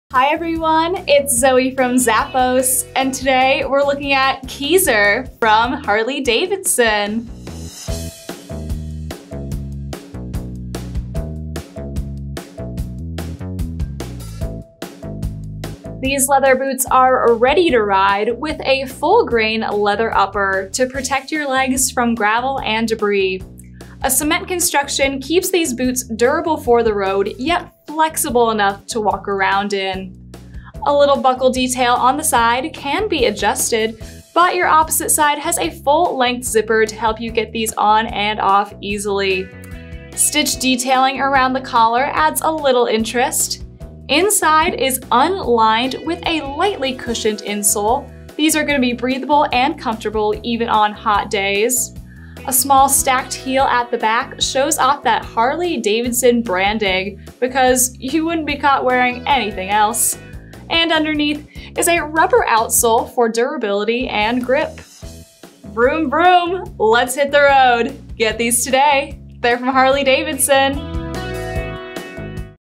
Harley-Davidson Keyser SKU: 9271551
The versatile Harley-Davidson&reg; Keyser leather boot is the perfect touch when dressing up or down.
Adjustable buckle with a YKK&reg; locking inside zipper.
Cement construction.
Unlined.
Durable rubber outsole.
Heel Height: 1.25".
Imported.